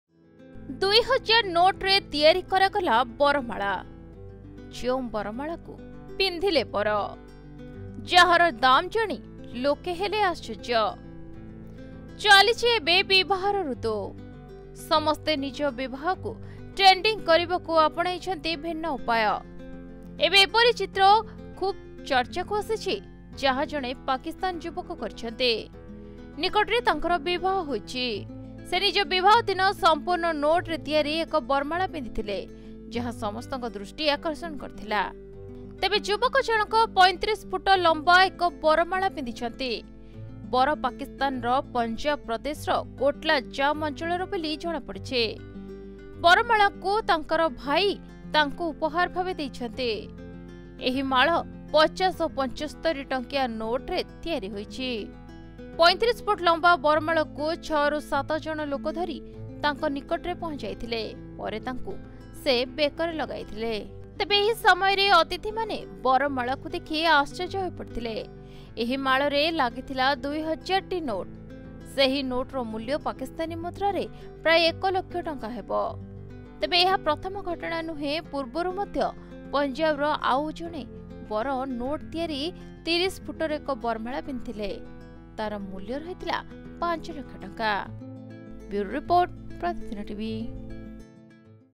୨୦୦୦ ଟଙ୍କିଆ ନୋଟ୍ ବରମାଳା ପିନ୍ଧି ବାହାହେଲେ ପାକିସ୍ତାନୀ ବର। Pakistani Man wears 35 feet long Cash garland
୨୦୦୦ ଟଙ୍କିଆ ନୋଟ୍ ବରମାଳା ପିନ୍ଧି ବାହାହେଲେ ପାକିସ୍ତାନୀ ବର। Pakistani Man wears 35 feet long Cash garland worth 1lakh.